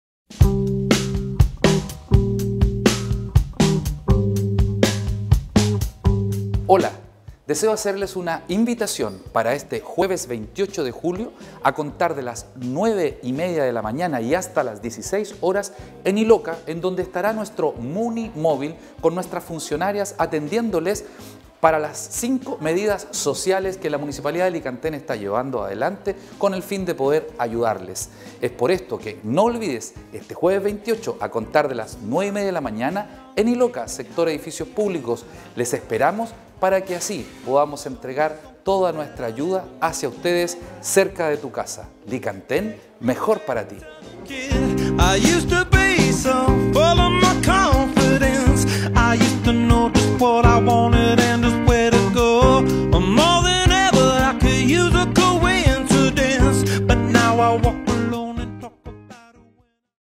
MUNICIPALIDAD EN TERRENO EN ILOCA ¡TE ESPERAMOS!
La Municipalidad de Licantén informa a la comunidad que este jueves 28 de julio, nos encontraremos en terreno, en el sector de la costa para resolver sus dudas sobre las 5 nuevas políticas sociales municipales.
📍Edificios Públicos (A un costado del cajero automático).
⏰09:00 a 16:00 horas.
¡Los esperamos!